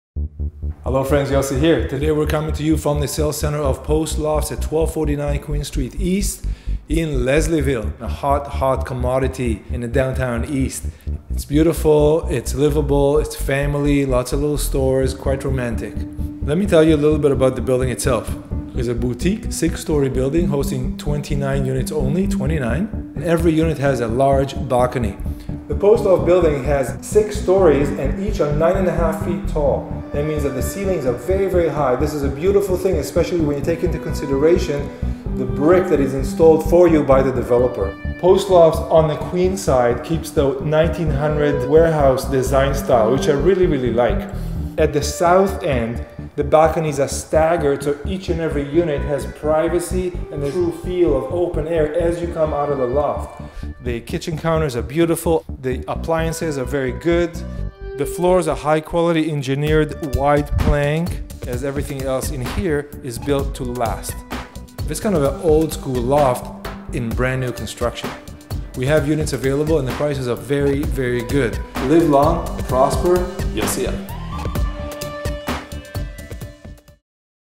Post Lofts Review | #107
Post Lofts @ 1249 Queen East is a true boutique loft development hosting 29 units. Price Per Foot (PSF), over all quality and appliance level beats any other similar development by a mile.

Post Lofts standard features are excellent. Each unit include:

- 9.5' high ceilings
- Spacious balconies
- Feature Brick Wall
- European Kitchen
- Gas Stove
- BBQ Gas Line on Balcony

About
Yossi Kaplan, MBA is a Toronto expert Realtor, specializing in Real Estate Investments, Pre-Construction, Assignments and MLS Listings.